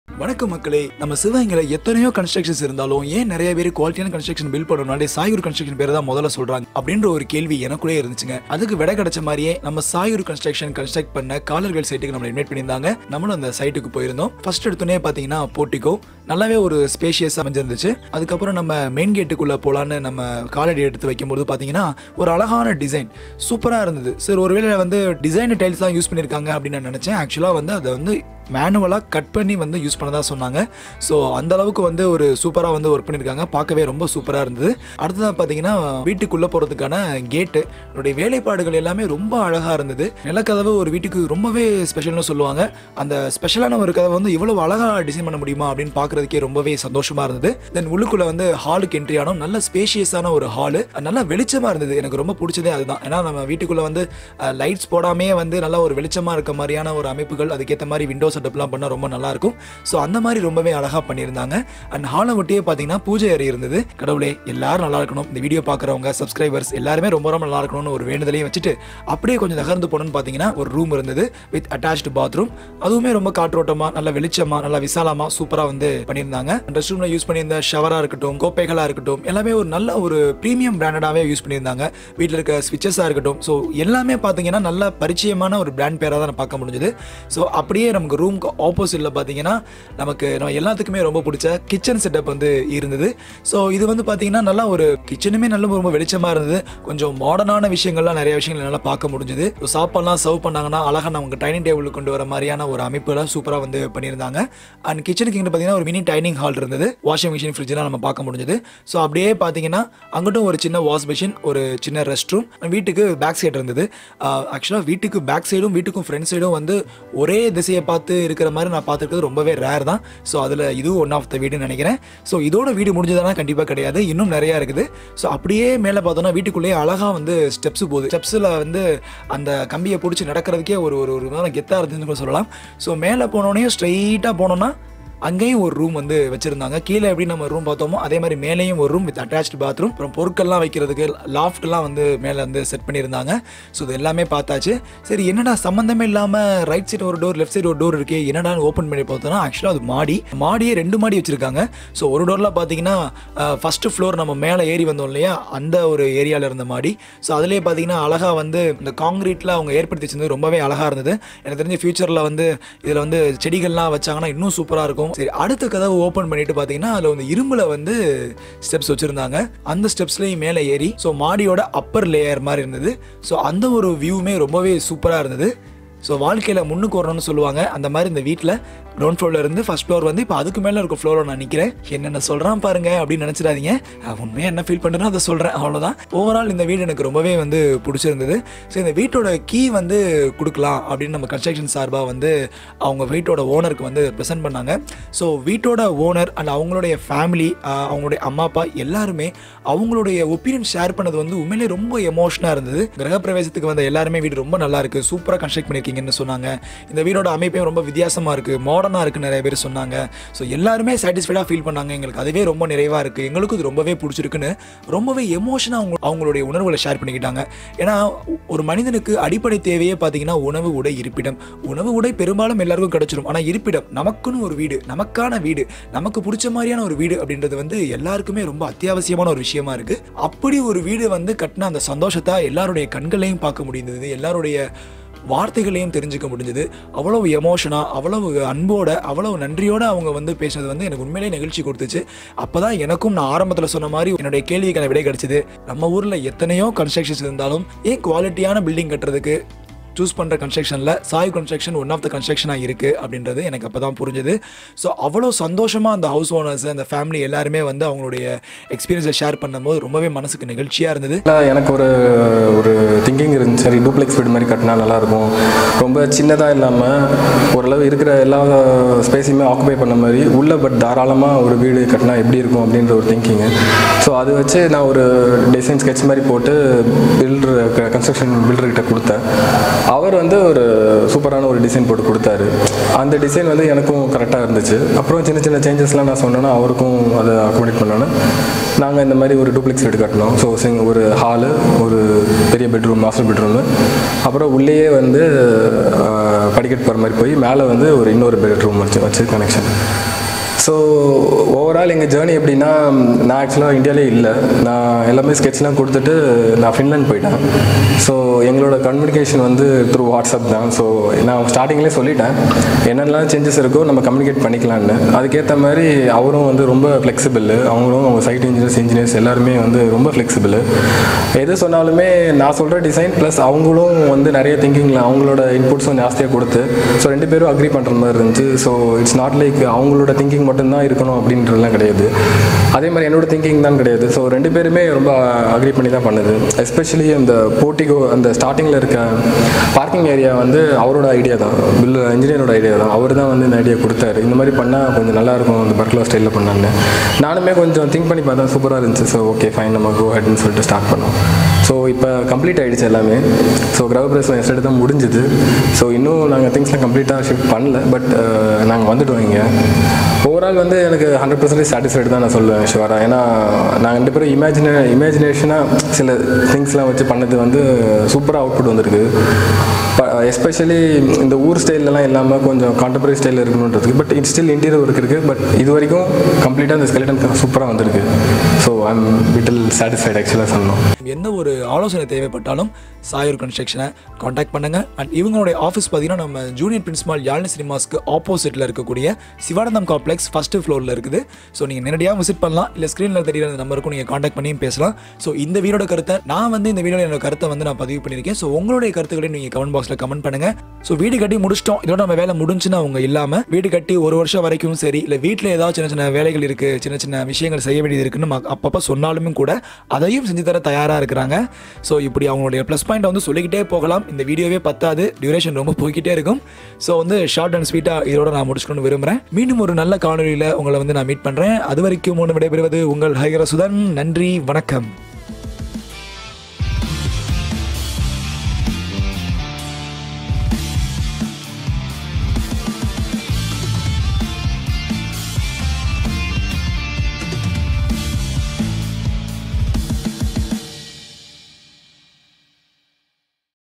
Sai Guru Constructions Kalayarkoil Site Visit & Review |+ 91 95511 09511 | Trending Sivagangai
உங்கள் கனவு இல்லங்களை தரமான பொருட்களையும் அனுபவமிக்க சிறந்த பணியாளர்களையும் கொண்டு உங்கள் எண்ணத்திற்கேற்ப வீடுகளை சிறந்த முறையில்  கட்டித்தருகிரார்கள் உங்கள் சாய் குரு கன்ஸ்ட்ரக்ஷன் & டெவலப்பர்ஸ் நிறுவனம் ...

Office Address :
Er.B.Jayapraksh
Sai Guru Construction & Developers,
First Floor,Sivanandham Complex,Opposite to Junior Prince Mall,Sivagangai.